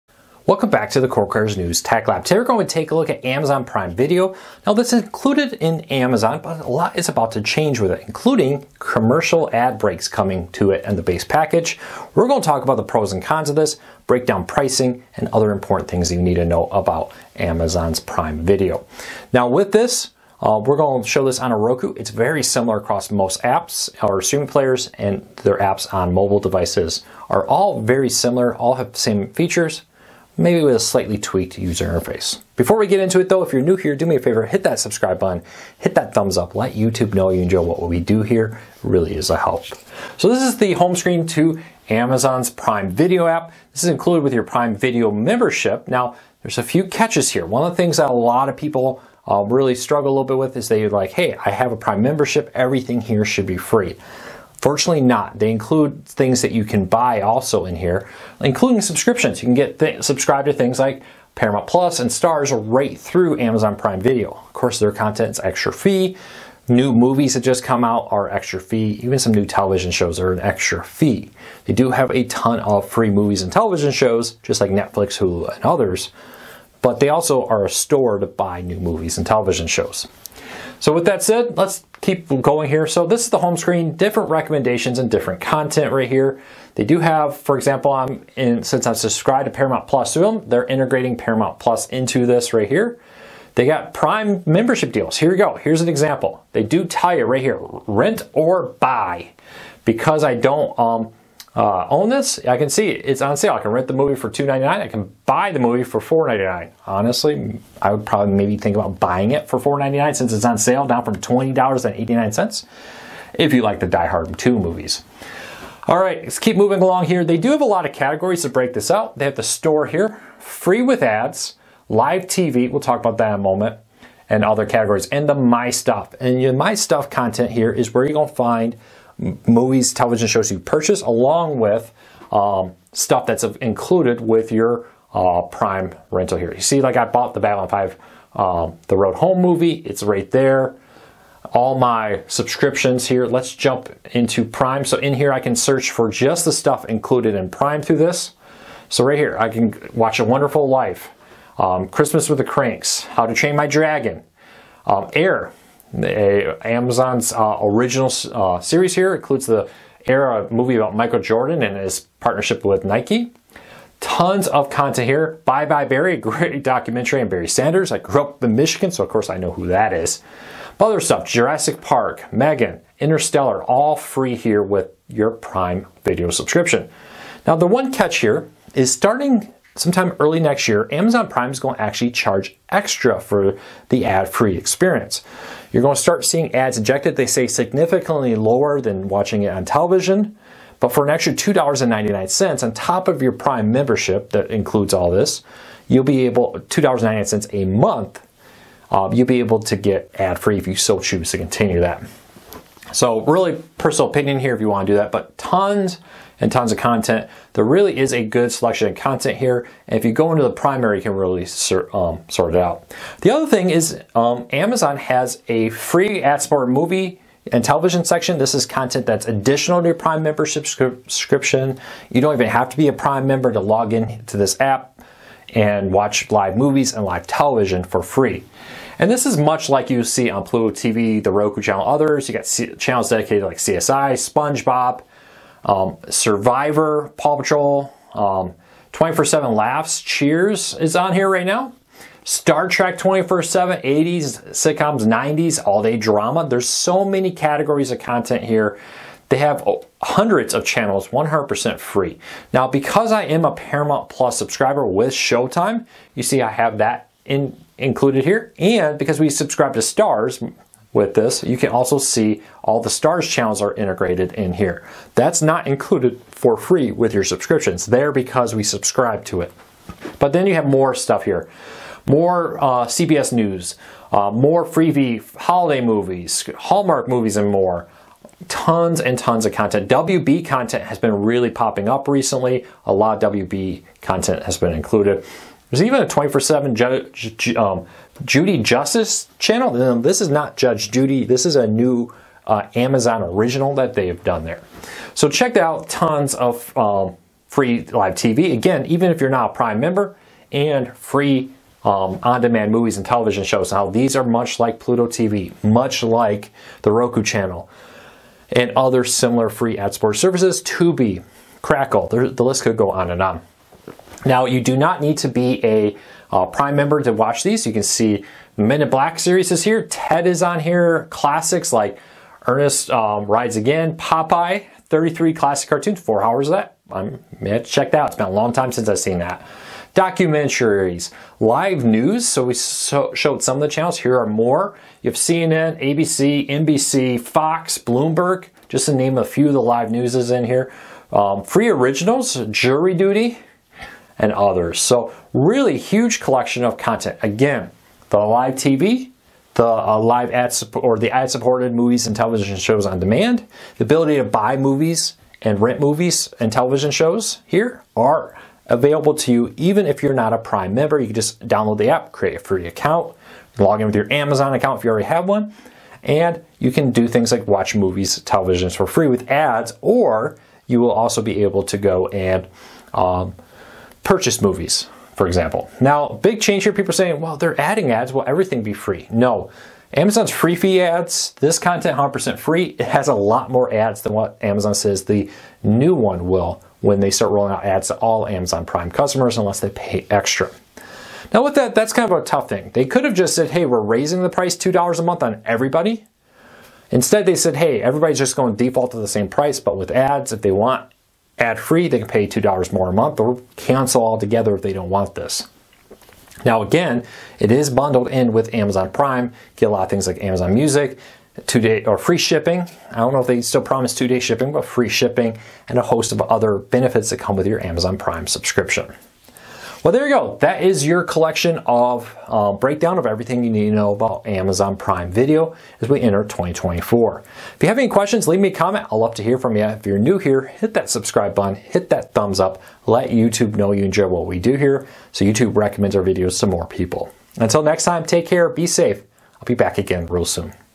Everything You Need to Know About Amazon Prime Video - Pricing, Free Content, Guide, & More
Today we take a look at Amazon Prime Video and answer your most common questions.

Did you know we have a Facebook group for cord cutting tech support?

Check out our Facebook Group dedicated to the best daily deals